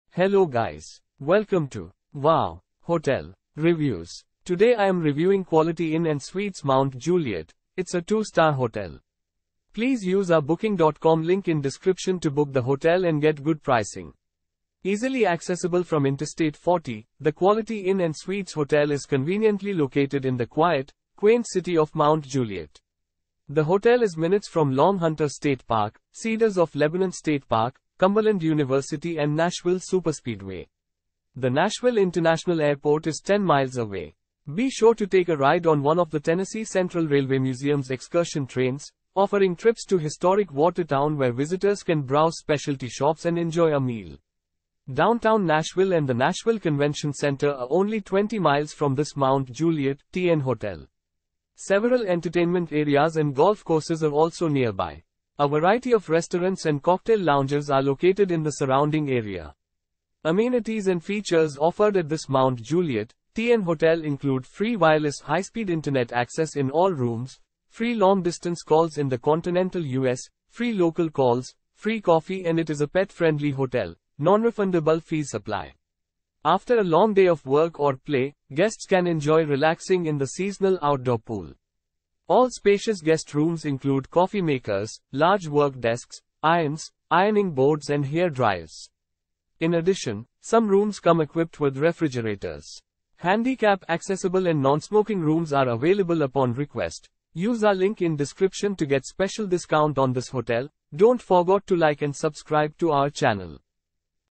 Quality Inn & Suites Mt. Juliet Review - Mount Juliet , United States of America 491755
Quality Inn & Suites Mt. Juliet

Easily accessible from Interstate 40, the Quality Inn & Suites hotel is conveniently located in the quiet, quaint city of Mt. Juliet. The hotel is minutes from Long Hunter State Park, Cedars of Lebanon State Park, Cumberland University and Nashville Superspeedway. The Nashville International Airport is 10 miles away.

Be sure to take a ride on one of the Tennessee Central Railway Museum's excursion trains, offering trips to historic Watertown where visitors can browse specialty shops and enjoy a meal. Downtown Nashville and the Nashville Convention Center are only 20 miles from this Mt. Juliet, TN hotel. Several entertainment areas and golf courses are also nearby. A variety of restaurants and cocktail lounges are located in the surrounding area.

Amenities and features offered at this Mt. Juliet, TN hotel include free wireless high-speed Internet access in all rooms, free long distance calls in the continental U.S., free local calls, free coffee and it is a pet-friendly hotel; nonrefundable fees apply.

After a long day of work or play, guests can enjoy relaxing in the seasonal outdoor pool.

All spacious guest rooms include coffee makers, large work desks, irons, ironing boards and hair dryers. In addition, some rooms come equipped with refrigerators. Handicap accessible and nonsmoking rooms are available upon request.